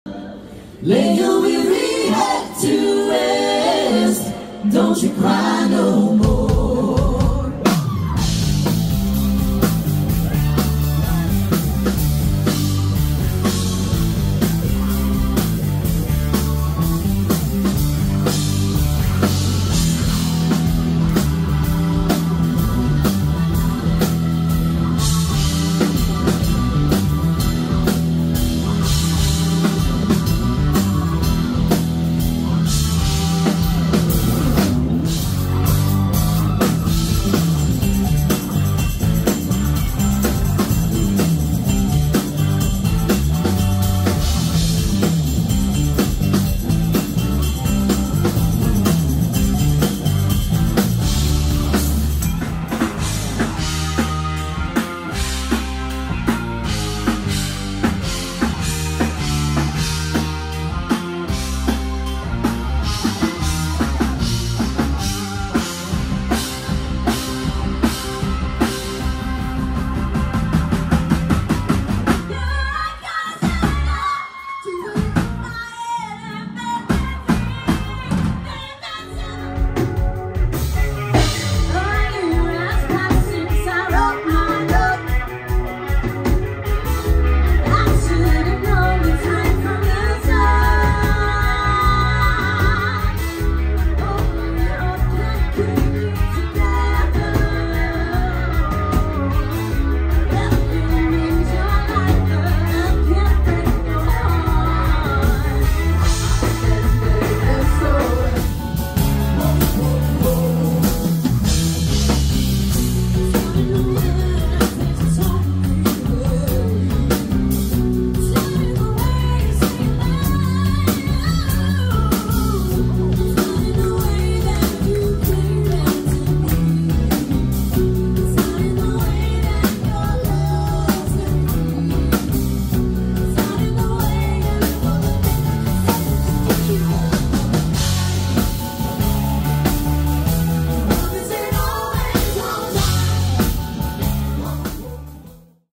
Benjamin Nurthen - Live Electric Guitar Reel
Hey there!
All clips taken from live performances in Syd Norman's Pour House on the Norwegian Breakaway!